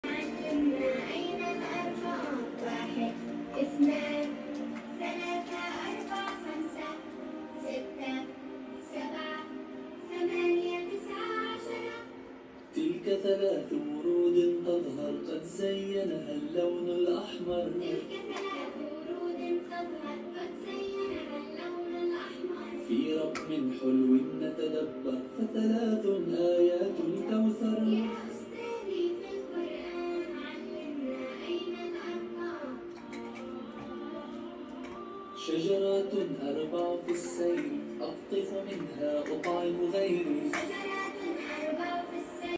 Sneak peek of 200 inches Projector. Kids love it.
This new  projector is amazing and I’m honoured to be one of the first reviewers here in YouTube.